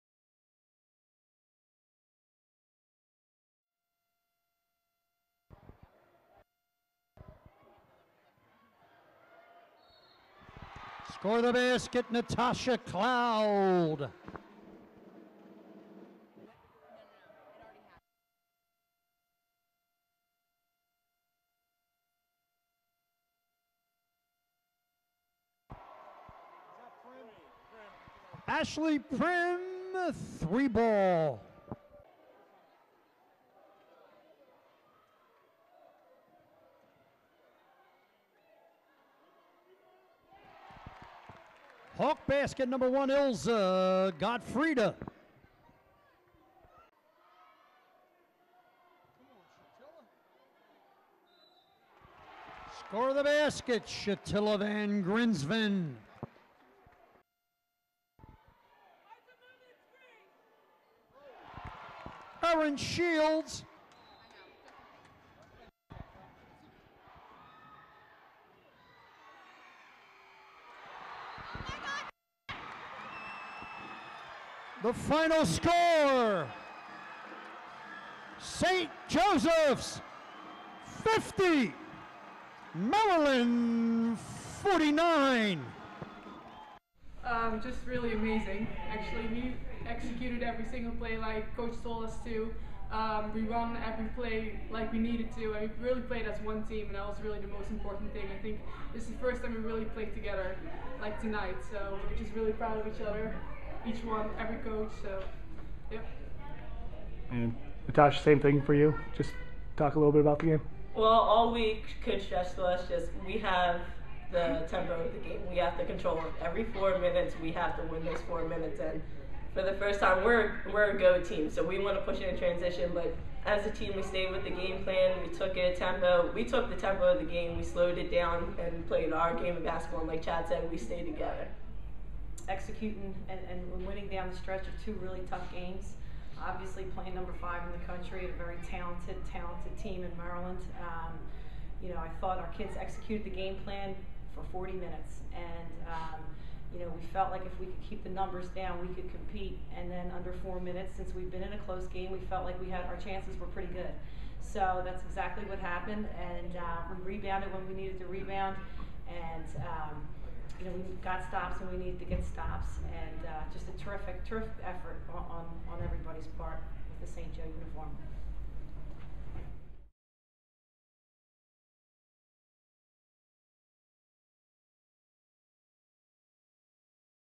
Saint Joseph's Women's Basketball: Hawks 50, #5 Maryland 49 - 11/17/12
The Saint Joseph's women's basketball team upset #5 Maryland, 50-49, Saturday, November 17, 2012 at Hagan Arena